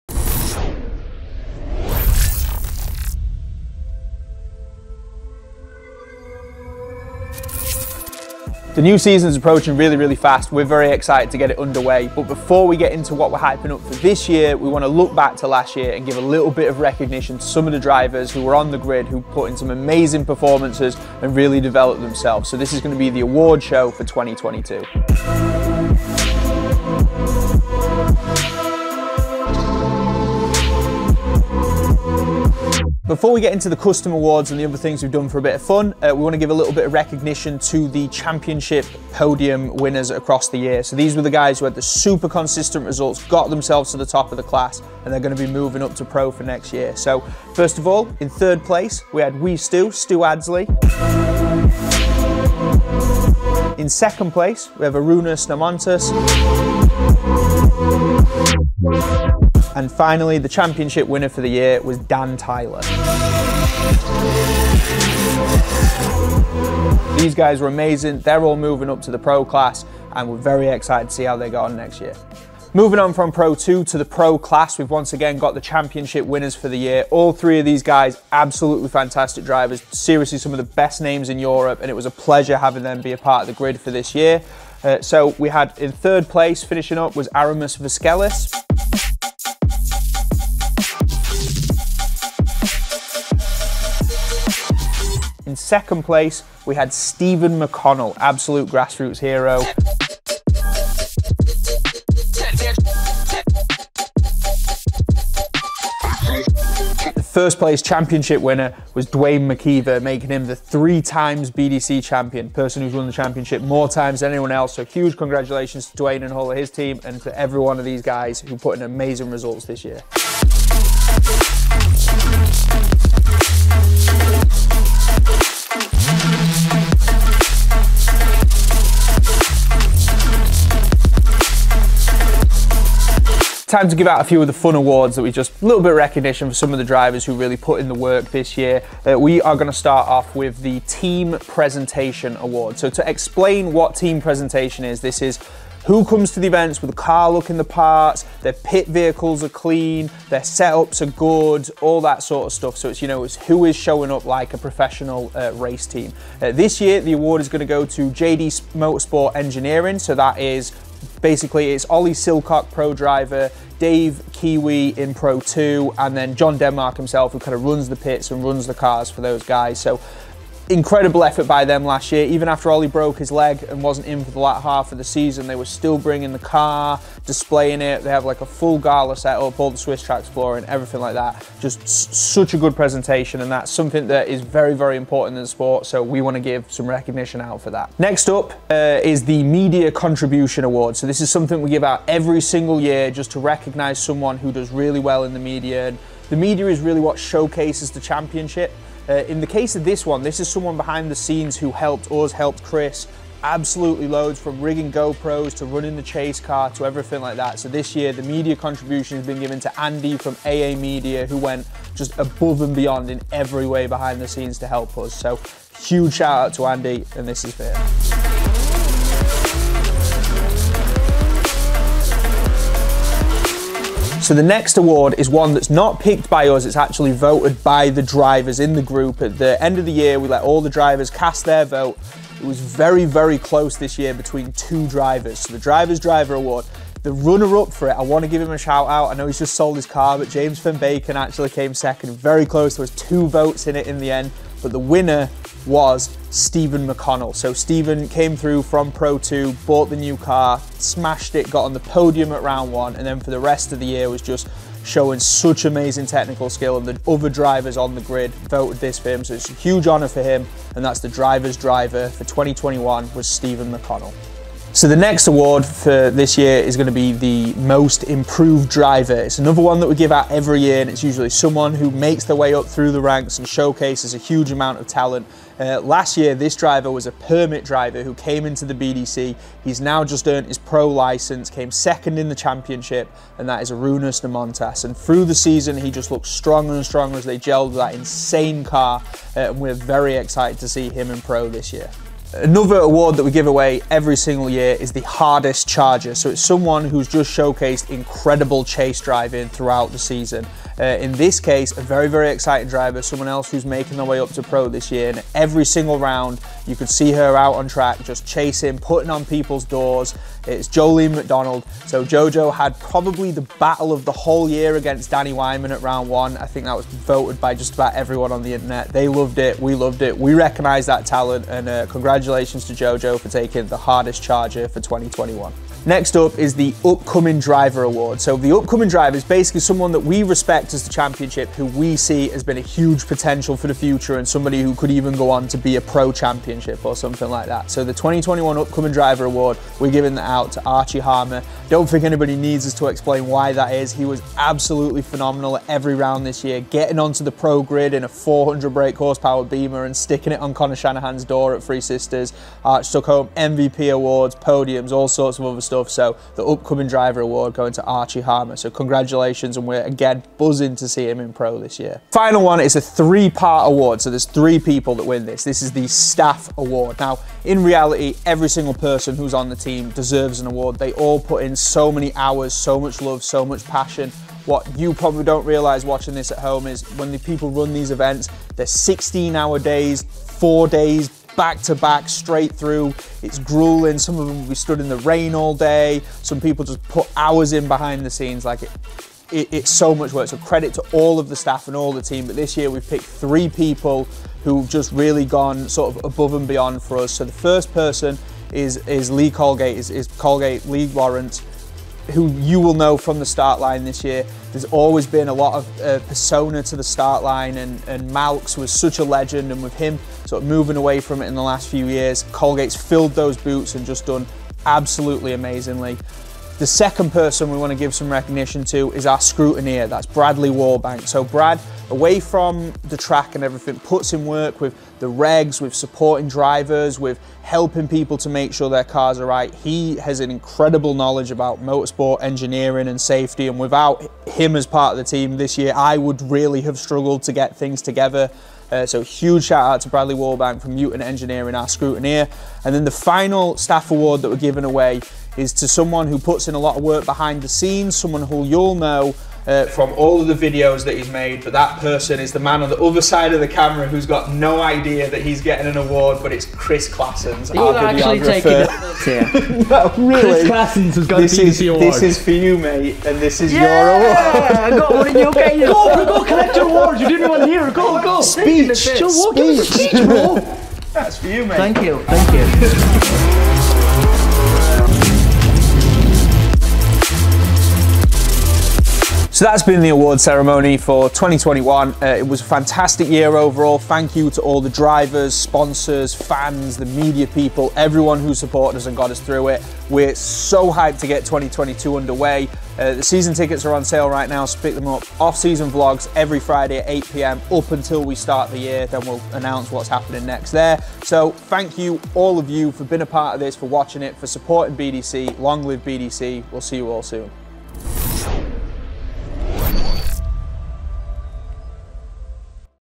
BDC 2021 Awards
We give out some recogniton to the drivers, media and staff who helped to make last year the amazing sucess is was!